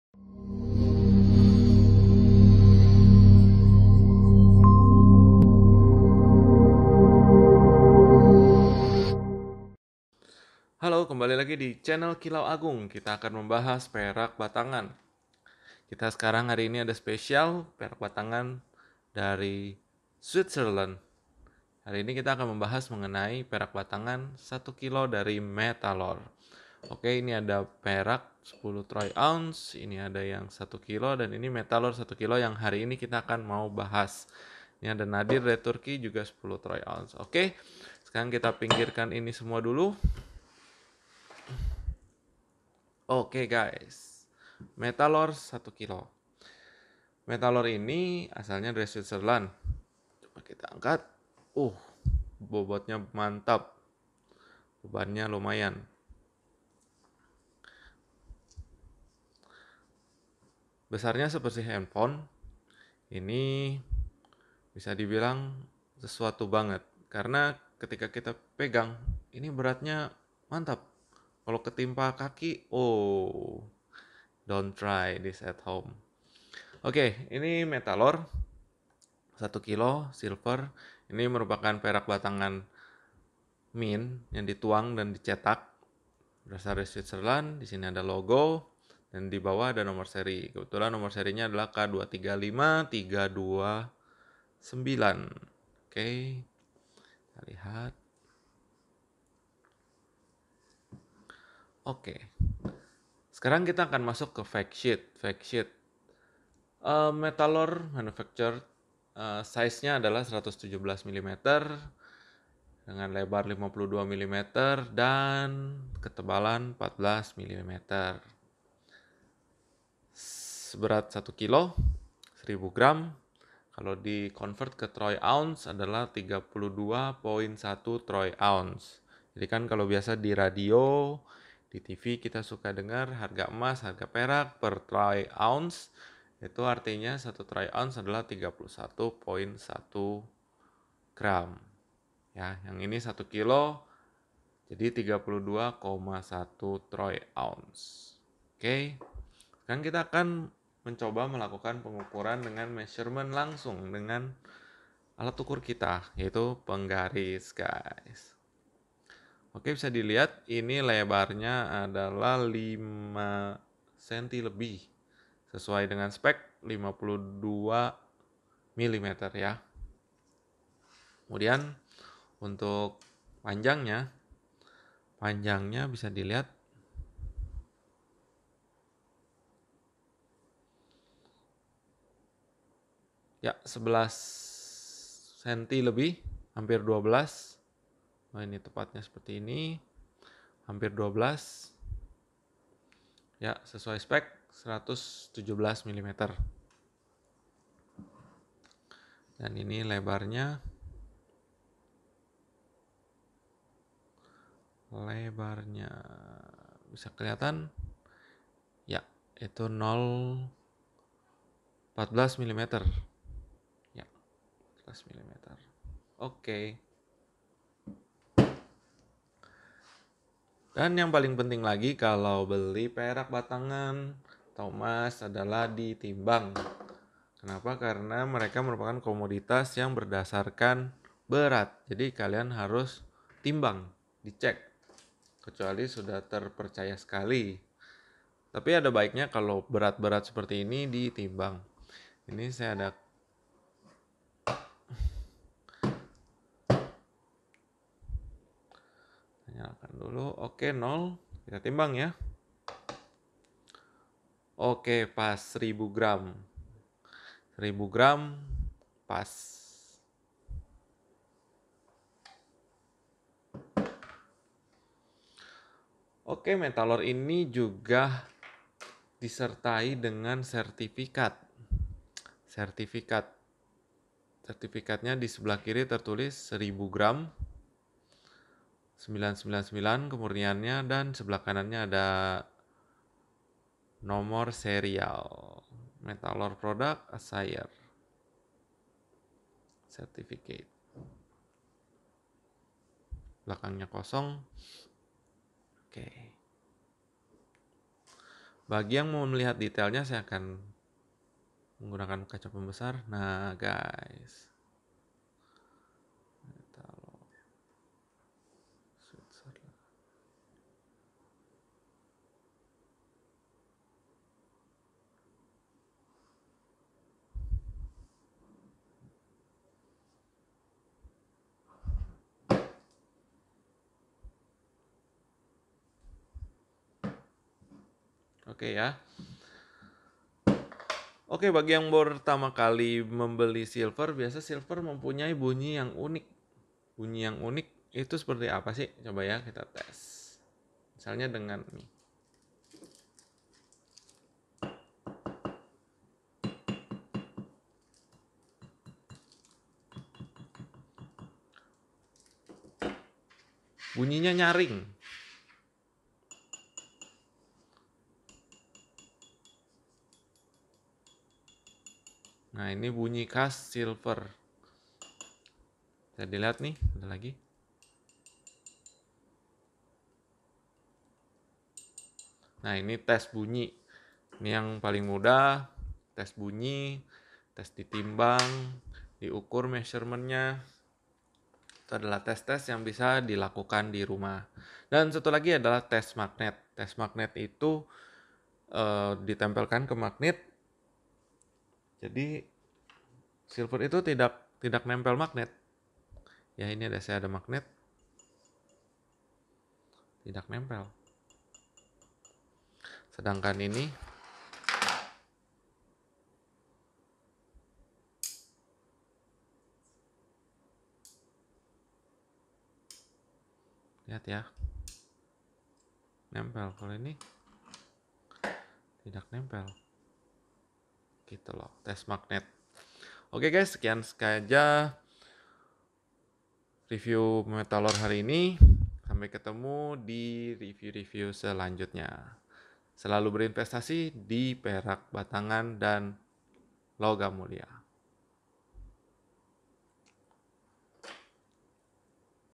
Perak Batangan Metalor swiss 1 Kilogram - silver bar. Investasi yang bagus
Investasi perak kini makin digemari di Indonesia. Padahal investasi ini sudah lama ada dari sejak dahulu kala. Di dunia international investasi perak merupakan investasi yang sangat populer selain Logam mulia (Emas)

Sekarang kita akan membahas mengenai perak batangan dari swiss.
perak ini dari pabrikan Metalor
Beratnya 1 Kilogram
dan merupakan produksi dari pabrik yang disertifikasi oleh LBMA (London Bullion Market Association)

kami akan sharing informasi investasi yang bisa bikin kalian cuan.